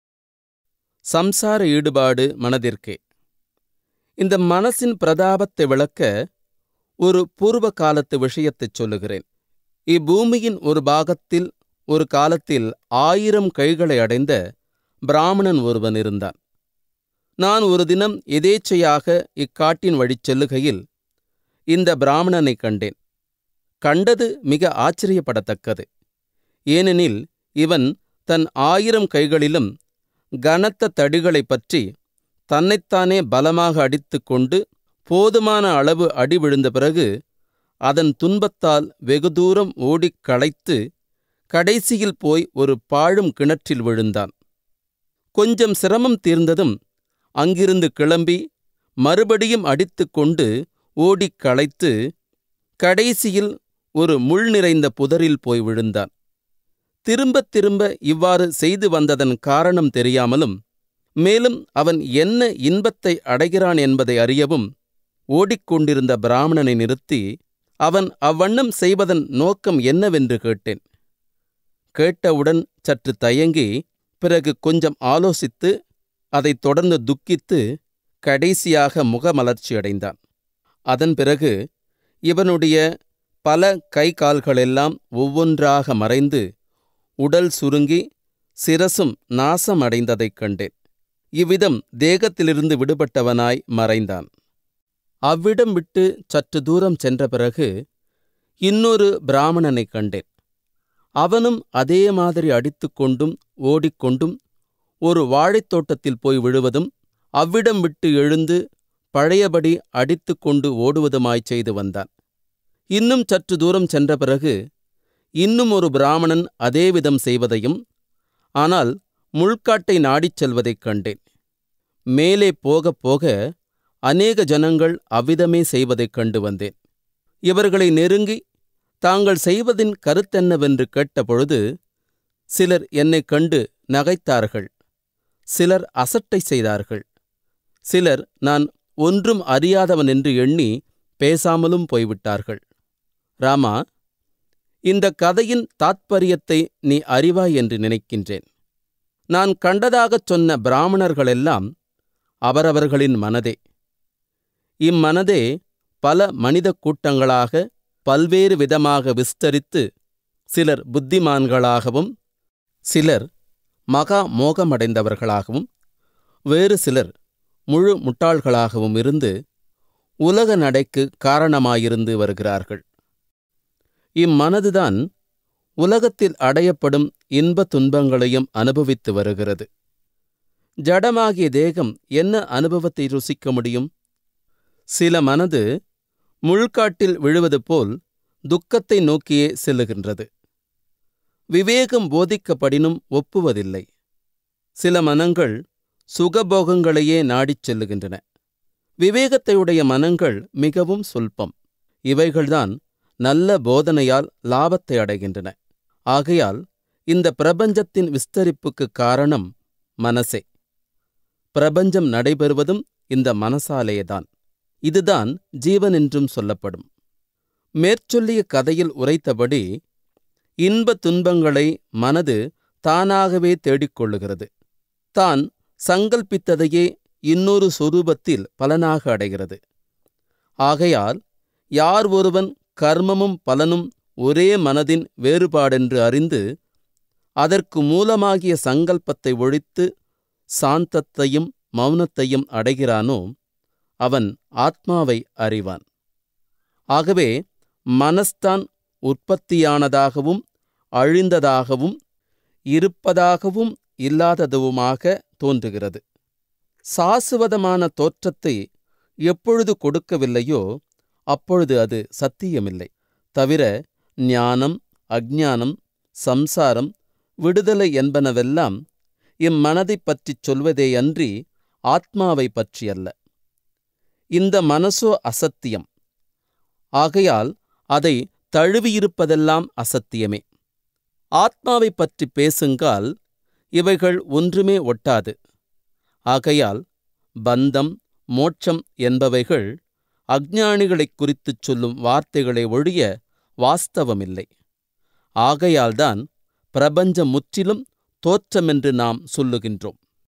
Yoga Vasistam Part - 28 I யோக வாசிஷ்டம் I சம்சார ஈடுபாடு மனதிற்கே.
ஞான வாசிஷ்டம் வசிஷ்ட மகரிஷி, ராமபிரானுக்கு உபதேசித்த ஞானப் பெட்டகம்.

மனிதனாக பிறந்த ஒவ்வொருவருக்கும் ஞான புரிதல் என்பது சாத்தியம்.
நாம் ஒவ்வொருவரும் சரியான பாதையில், சரியான குருவின் துணையோடு வழிநடத்தப் பட்டால் அனைவருக்கும் இந்த ஞானத் தெளிவு 100% சாத்தியம்.

ஞான தெளிவு பெற விரும்பும் அன்பர்கள் இந்த ஞான சத் சங்கத்தினை பயன்படுத்திக் கொள்ளுமாறு கேட்டுக்கொள்கிறோம்.